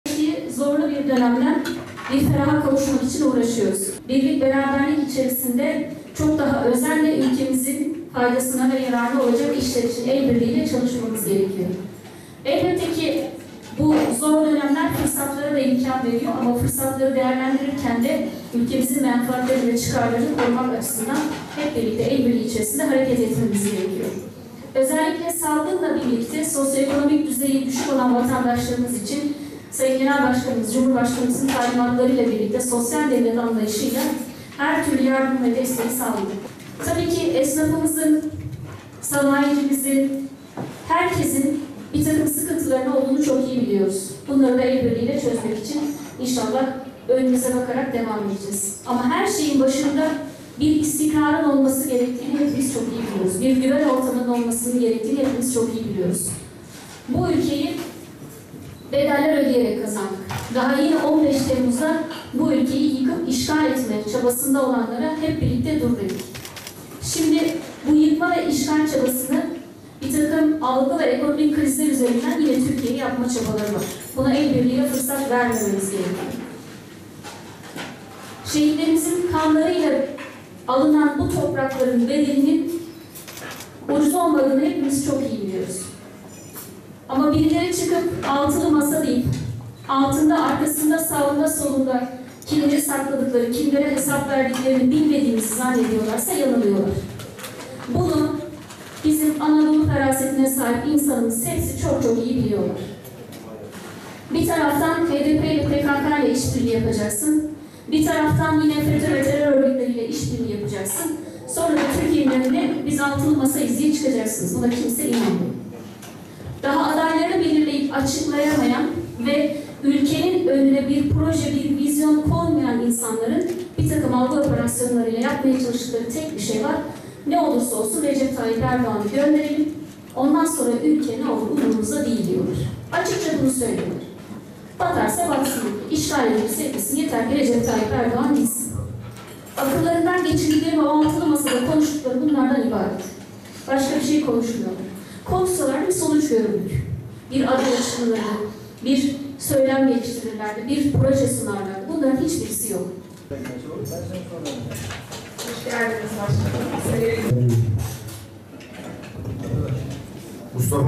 AK Parti Genel Başkan Yardımcısı Usta, STK temsilcileriyle buluştu
AK Parti Genel Başkan Yardımcısı Leyla Şahin Usta, muhalefeti eleştirerek, "Bir taraftan HDP ve PKK ile iş birliği yapacaksın, bir taraftan yine FETÖ ve terör örgütleriyle iş birliği yapacaksın, sonra da Türkiye'nin önüne 'Biz 6'lı masayız' diye çıkacaksınız, buna kimse inanmıyor." dedi.
Usta, sivil toplum kuruluşu temsilcileriyle Beyşehir Öğretmenevi'nde bir araya geldiği toplantıda, Türkiye ve dünyanın geçtiği zorlu bir dönemde ülkeyi feraha kavuşturmak için uğraş verdiklerini söyledi.
Ülkenin faydasına olacak işler için birlik ve beraberlik içerisinde çalışma yapılması gerektiğini vurgulayan Usta, sosyal devlet anlayışıyla aileye destek olacak çalışmaları artırdıklarını dile getirdi.
Esnaf ve sanayicilerin sıkıntılarının farkında olduklarını anlatan Usta, şöyle konuştu:
"Bunları da el birliğiyle çözmeye devam edeceğiz. Ama her şeyin başında bir istikrar ve güven ortamı olmasının gerektiğini hepimiz çok iyi biliyoruz. Bu ülkeyi bedeller ödeyerek kazandık. 15 Temmuz'da bu ülkeyi yıkıp işgal etme çabasında olanlara hep birlikte 'dur' dedik. Şimdi bu yıkma ve işgal çabasını birtakım algı ve ekonomik krizler üzerinden yine Türkiye'ye yapma çabaları var. Buna el birliğiyle fırsat vermememiz gerekiyor. Şehitlerimizin kanlarıyla alınan bu toprakların bedelinin ucuz olmadığını hepimiz çok iyi biliyoruz ama birileri çıkıp, '6'lı masa' deyip, altında, arkasında, sağında, solunda kimi sakladıklarını, kimlere hesap verdiklerini bilmediğimizi zannediyorlarsa yanılıyorlar."
Usta, Anadolu ferasetine sahip insanların yaşananların farkında olduğunu belirterek şunları kaydetti:
"Bir taraftan HDP ve PKK ile iş birliği yapacaksın, bir taraftan yine FETÖ ve terör örgütleriyle iş birliği yapacaksın, sonra da Türkiye'nin önüne 'Biz 6'lı masayız' diye çıkacaksınız, buna kimse inanmıyor. Daha adaylarını belirleyip açıklayamayan ve ülkenin önüne bir proje, vizyon koymayan insanların birtakım algı operasyonlarıyla yapmaya çalıştıkları tek bir şey var; 'Ne olursa olsun Recep Tayyip Erdoğan'ı gönderelim, ondan sonra ülke ne olur umurumuzda değil'. Açıkça bunu söylüyorlar. 'Batarsa batsın, işgal edilirse edilsin, yeter ki Recep Tayyip Erdoğan gitsin.' Akıllarından geçirdikleri ve 6'lı masada konuştukları bunlardan ibaret. Başka bir şey konuşmuyorlar. Konuşsalar bir sonuç görürdük, aday açıklanır, söylem geliştirir ve proje sunarlardı. Bunların hiçbirisi yok."
Usta, konuşmasının ardından STK temsilcilerinin taleplerini dinledi.
Programa, AK Parti Konya milletvekilleri Tahir Akyürek, Gülay Samancı, Orhan Erdem, Hacı Ahmet Özdemir, Ahmet Sorgun, AK Parti İl Başkanı Hasan Angı, AK Parti Beyşehir İlçe Başkanı Recep Elkin, belediye meclis üyeleri ve partililer katıldı. (aa)